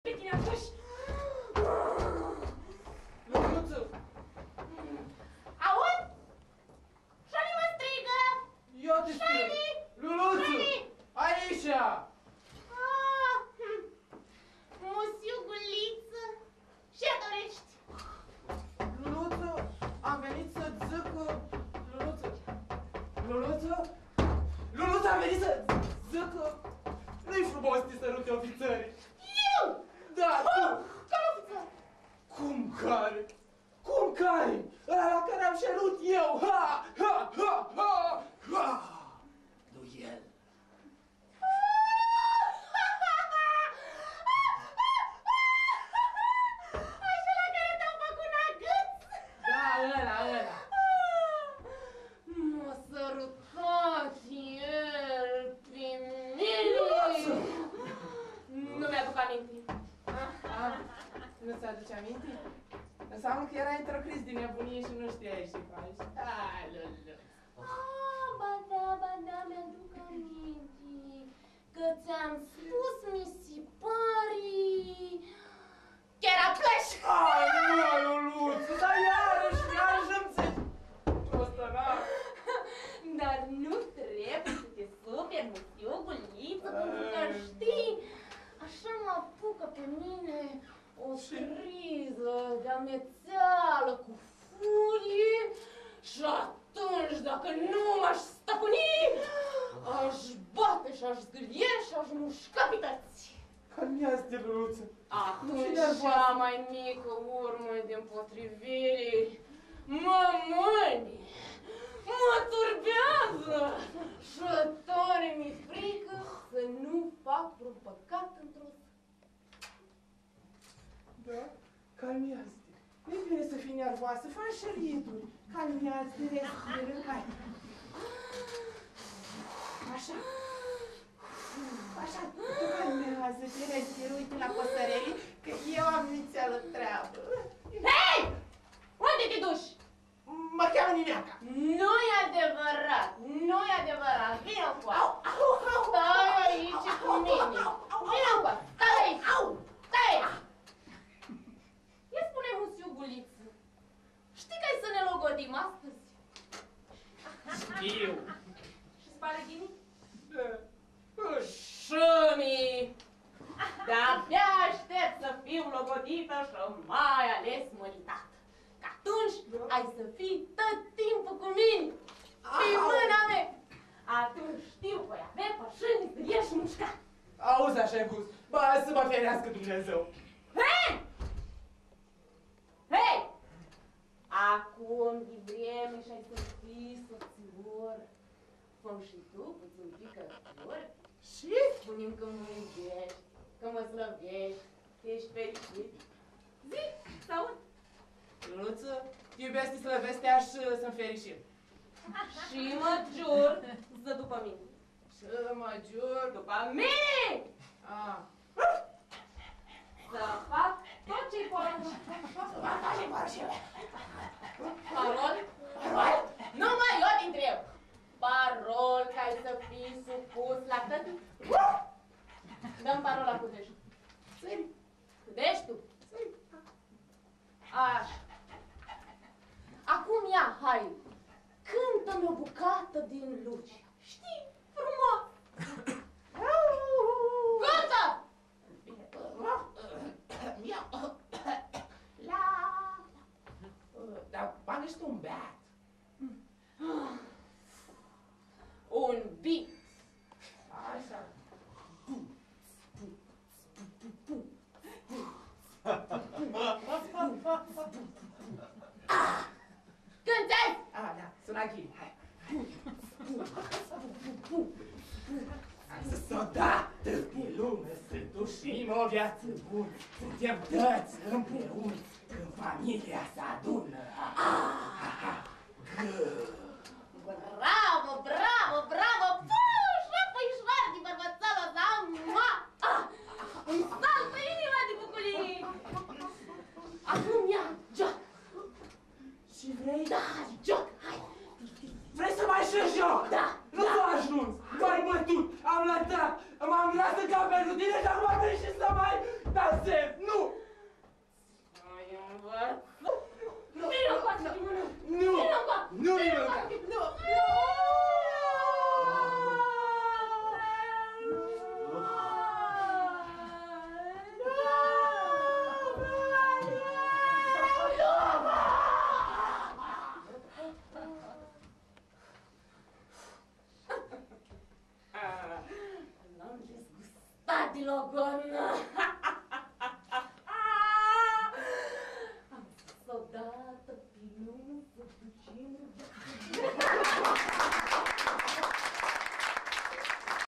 UNATC Examen Papusi anul I sem II
Adaptare dupa Chirita in provincie de Vasile Alecsandri.
Luluta: Ada Totolan
Gulita: Alexandru Neculcea